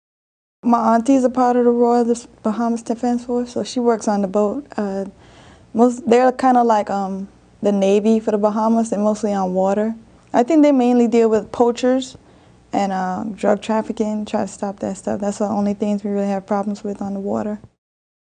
Military in the Bahamas
In this video, a Navy petty officer 3rd class explains the Bahamian military.